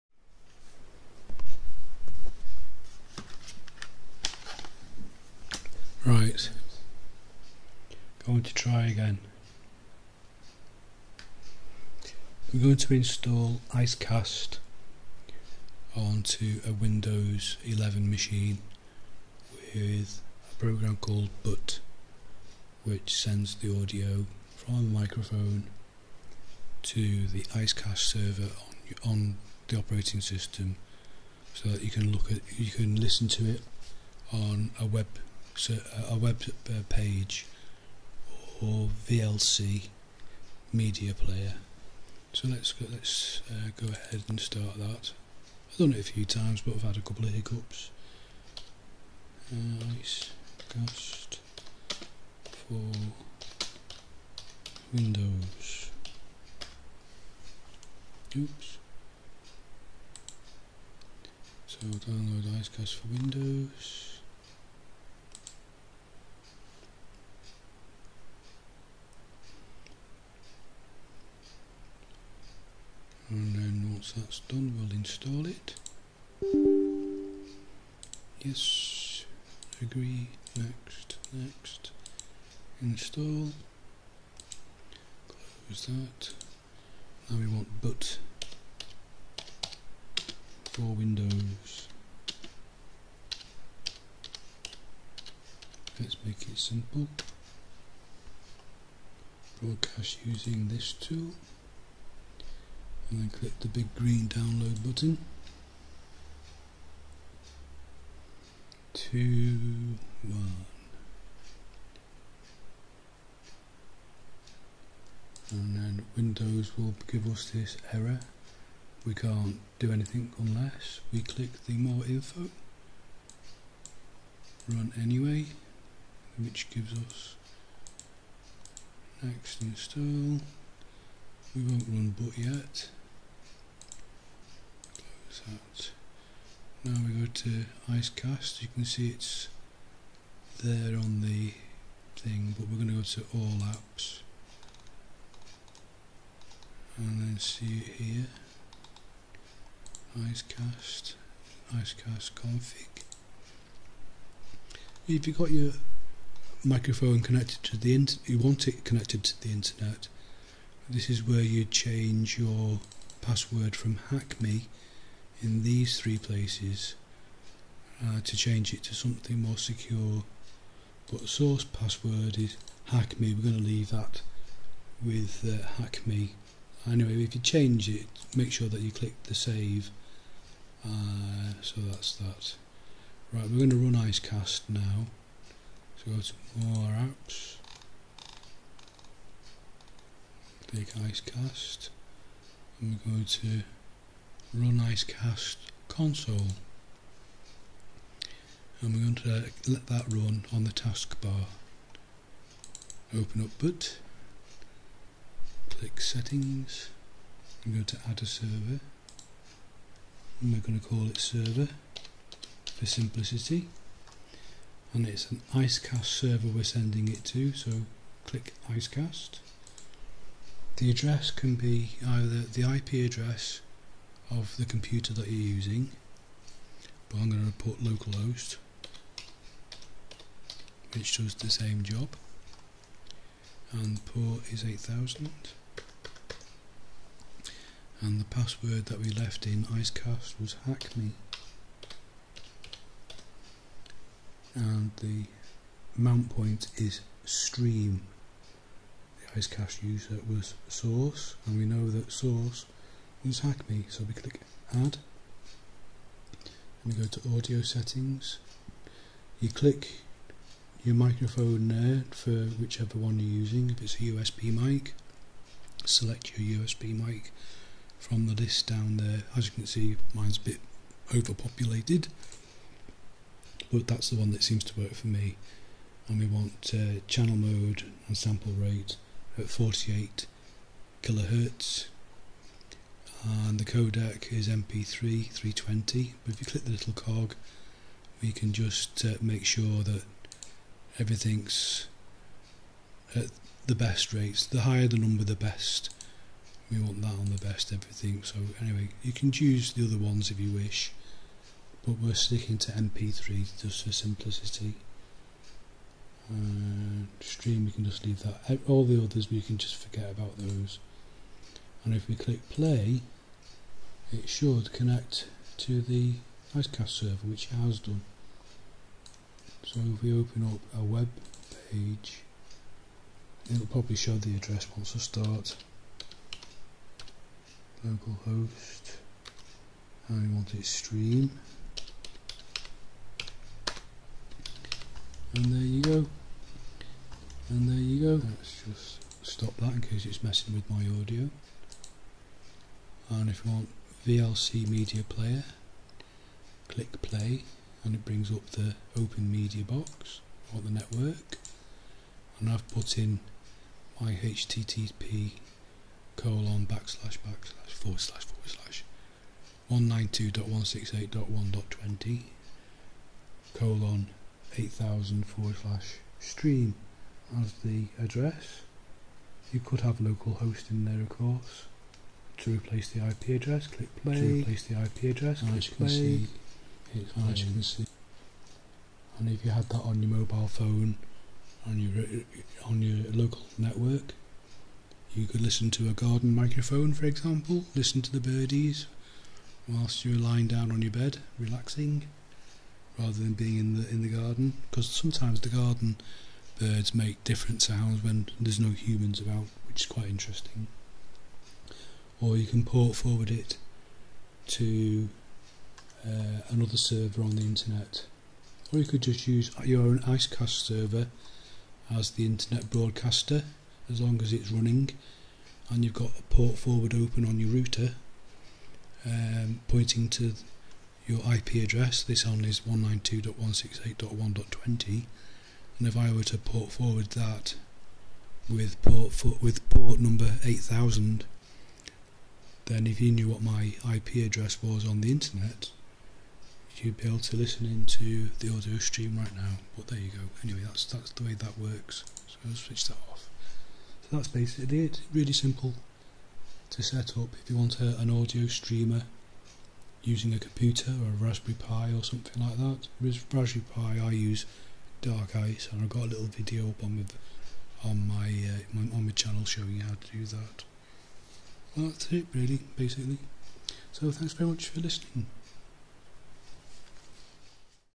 Installing Icecast and Butt onto a windows 11 machine
Simple video showing how to install Icecast and Butt on to Windows 11.
This is for streaming onto your local LAN

You need to port forward your Icecast servers address through your router to port 8000 for streaming to the Internet

The white noise is (I believe) from the AVerMedia EzRecorder330 which I was using to record the screen, I was plugged directly into my stereo mic input on my computer, with the mic setting switched to 'listen' to the input, so that the audio was sent over the HDMI cable to the recorder. Quite dissapointing now that I've heard it on my channel, I'll probably just use an external recorder next time to up the quality, but I'll have a play with the recorder again sometime to try and see whats going on with the white noise.